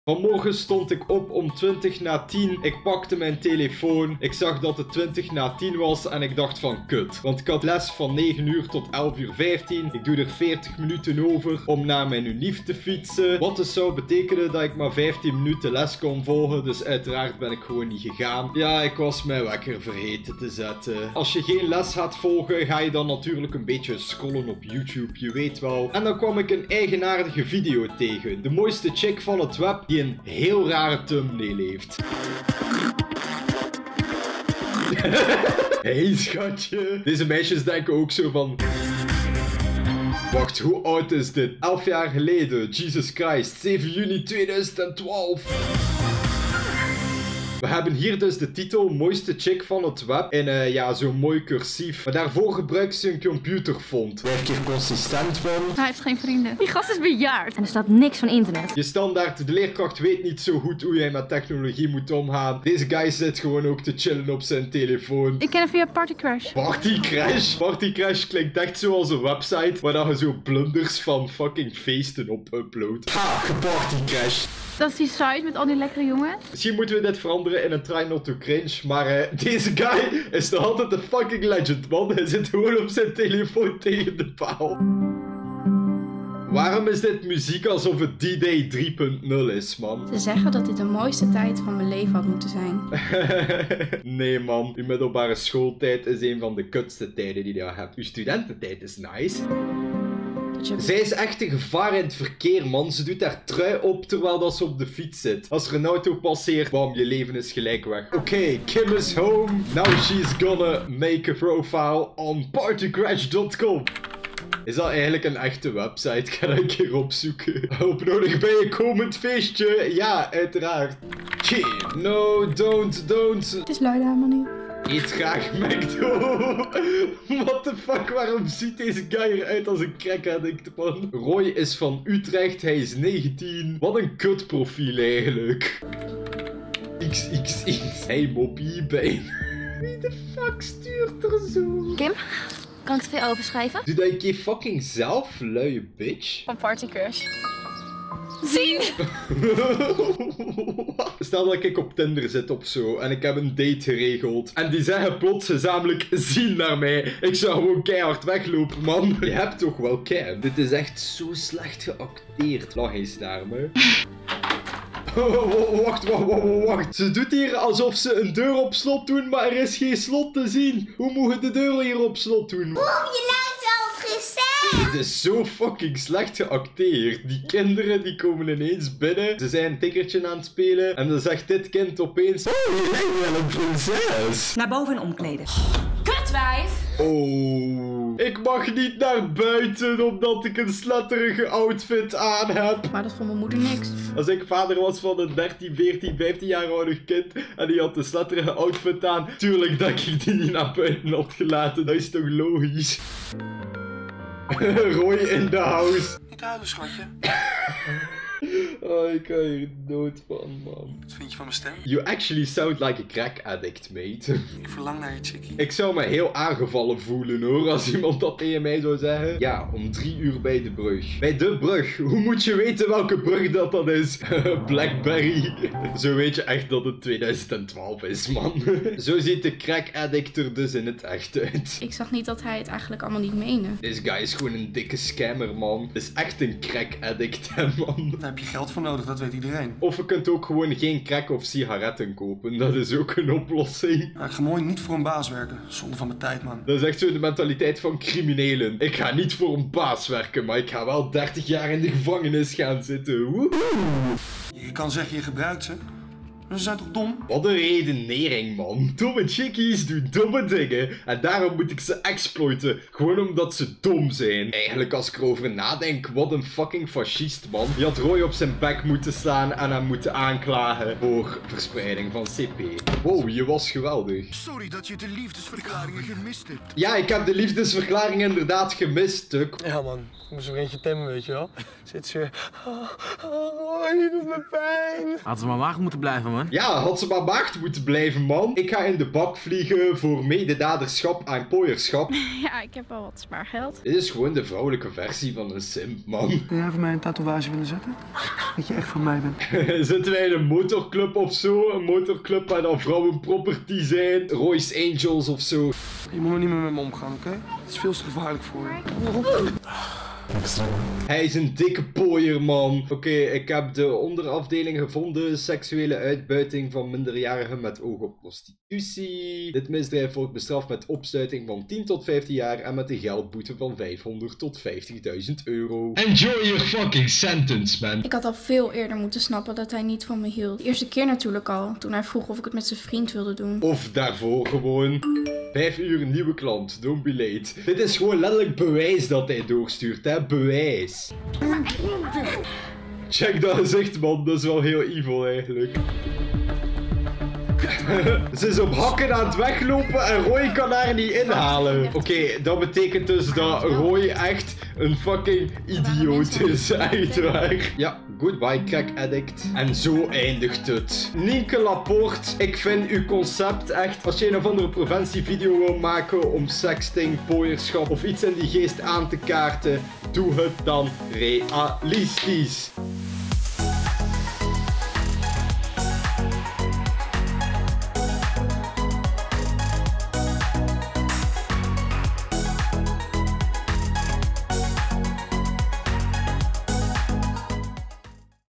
MOOISTE CHICK van het WEB !!!
In deze video reageer ik op 1 of andere rare preventievideo over sexting ofzo ?!